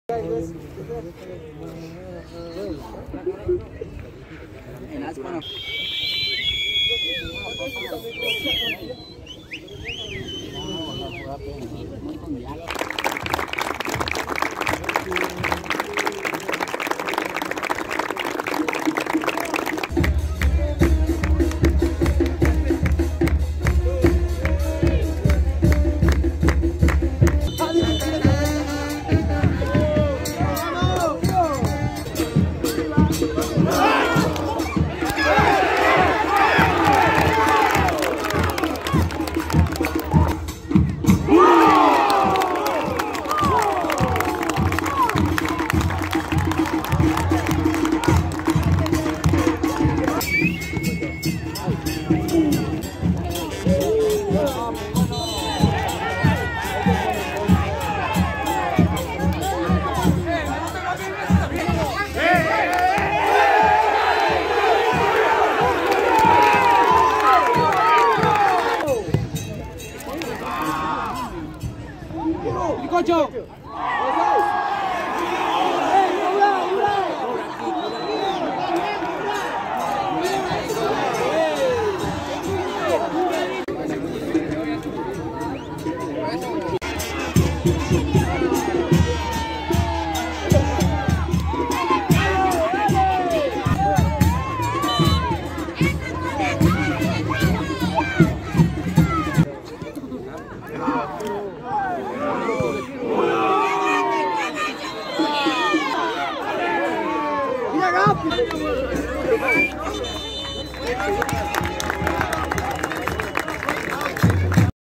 UNIÓN ACONQUIJA 1 - CENTRAL NORTE 1 - FECHA 1- TORNEO REPECHAJE - PRIMERA - LTF
Ezequiel Valdez (U)⚽
Ezequiel Rossi(CN)⚽